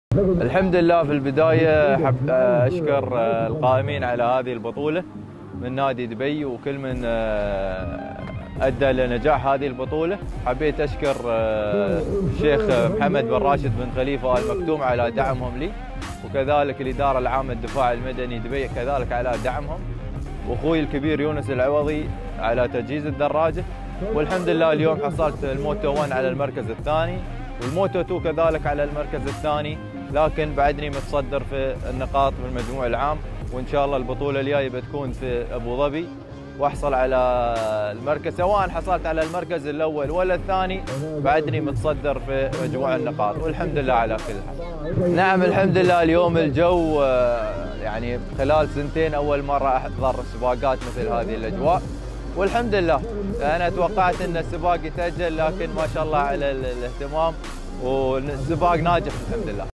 Amer Abdelrazzaq Hawair - UAE Int'l Aquabike Championship - Heat 6
02.03.19
عامر عبد الرزاق حوير
Amer Abdelrazzaq Hawair
Runabout GP3 (2nd Place)

بطولة الإمارات الدولية للدراجات المائية - الجولة 6
UAE Int'l Aquabike Championship - Heat 6